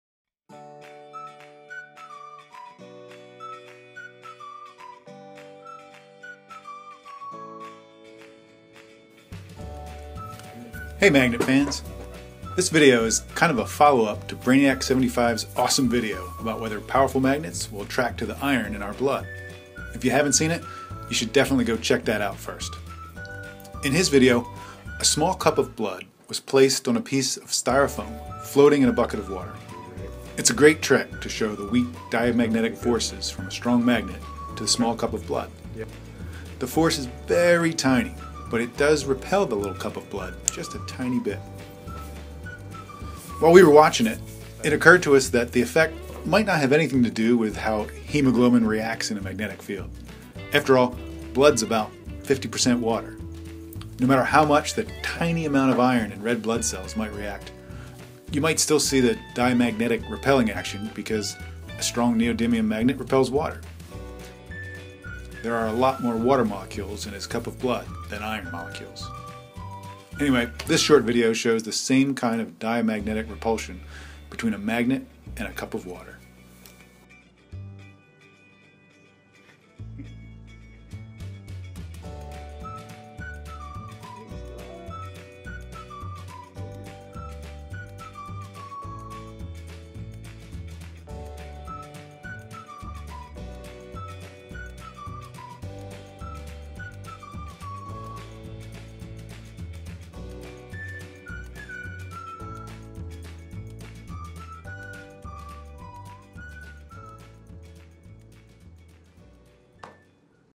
Is water magnetic??
Actually, it's diamagnetic! This is partly in response to Brianiac75's video about if the iron in our blood is attracted to magnets.